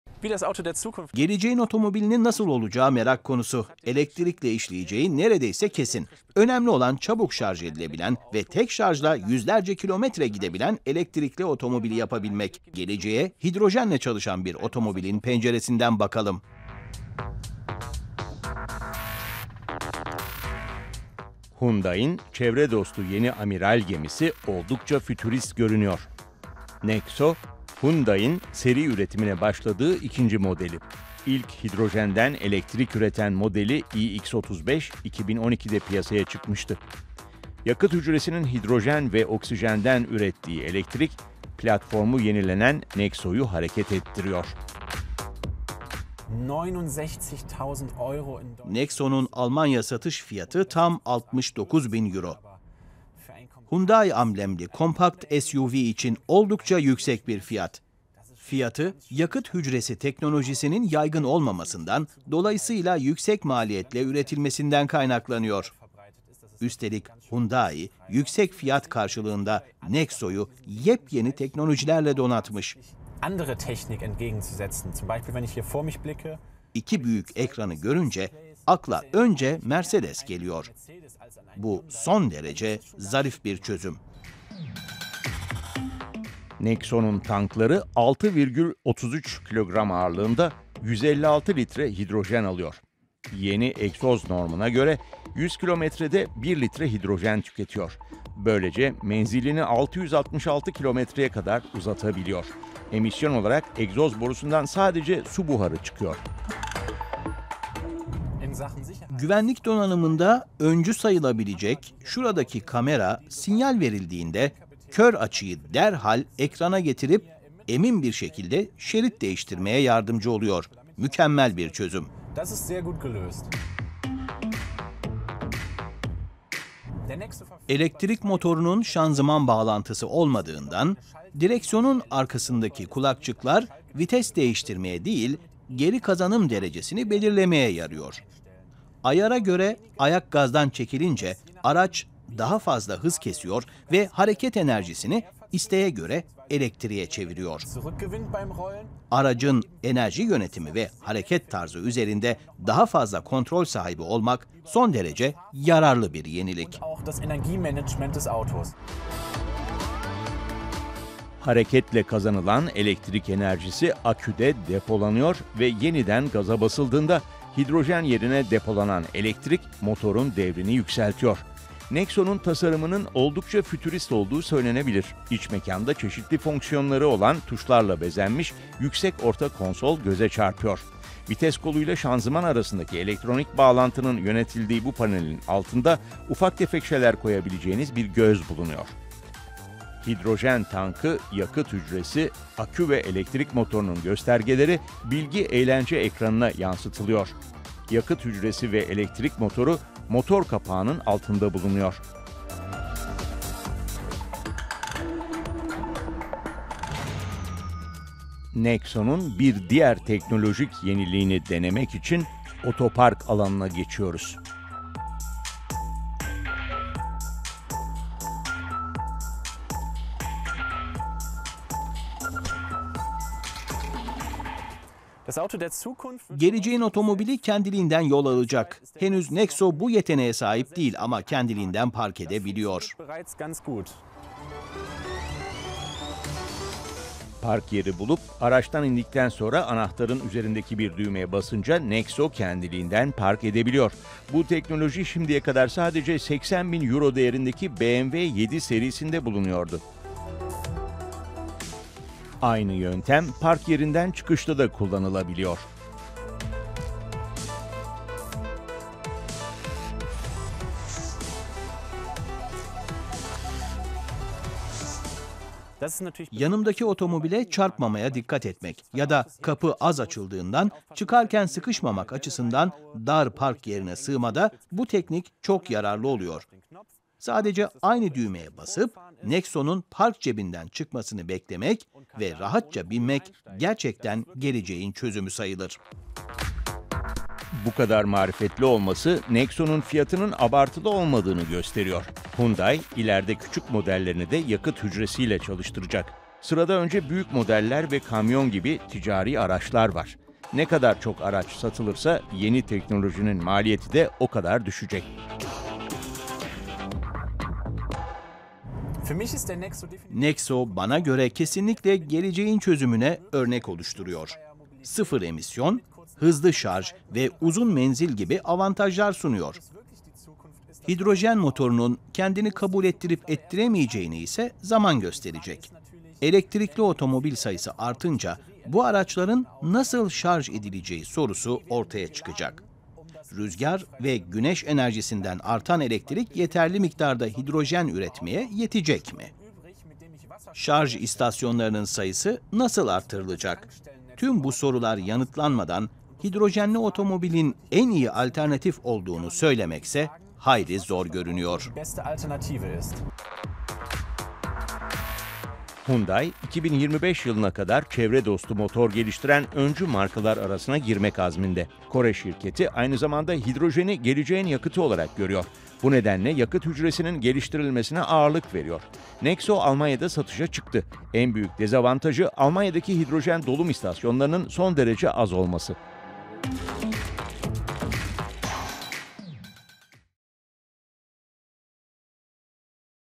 Hyundai’nin çevre dostu yeni amiral gemisi - DW Türkçe
Kısa şarjla uzun yol kat edebilecek çevre dostu motor nasıl olmalı? Hyundai’nin çözümü Nexo adını taşıyor. Hidrojen yakıtlı NEXO yeni tüketim normuna göre tek dolumla 666 kilometre gidebiliyor.